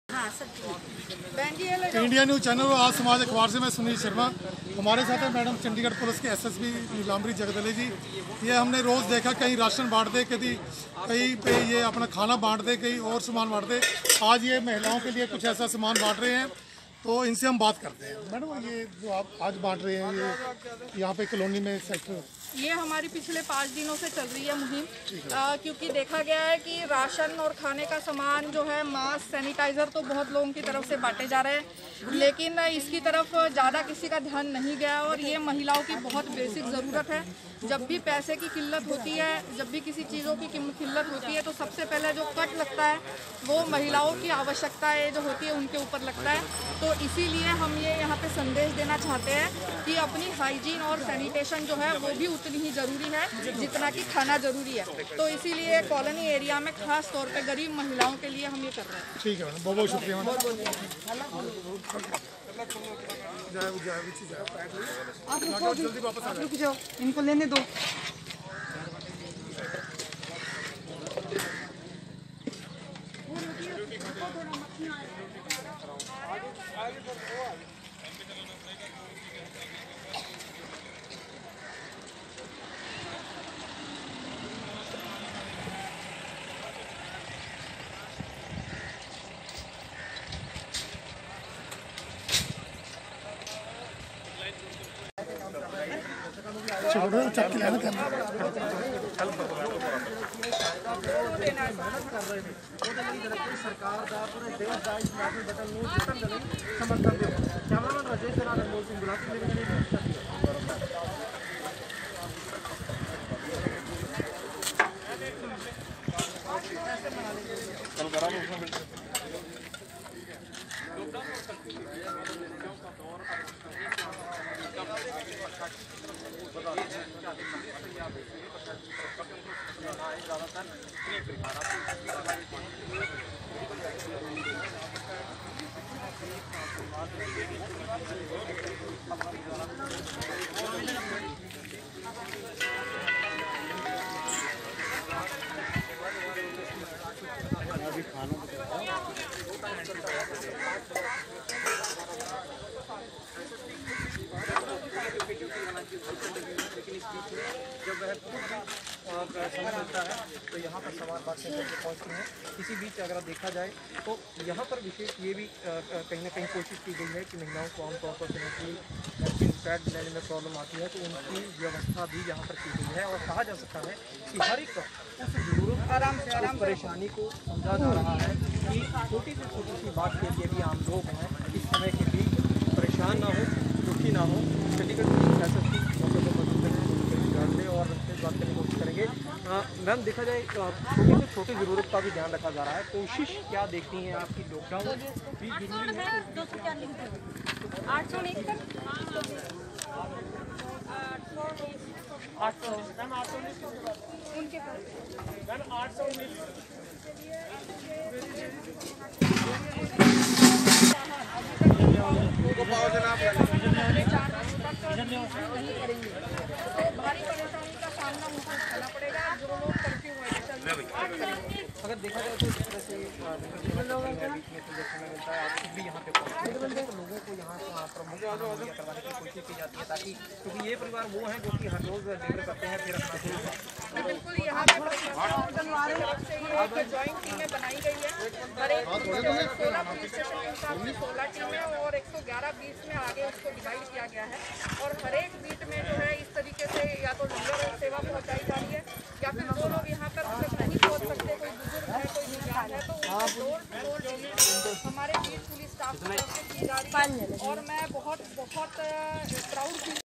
चंडीगढ़ पुलिस की SSP पहुंची सेक्टर 52 कॉलोनी लोगों को बांटा लंगर
India news channel Se Sunil Sharma ki report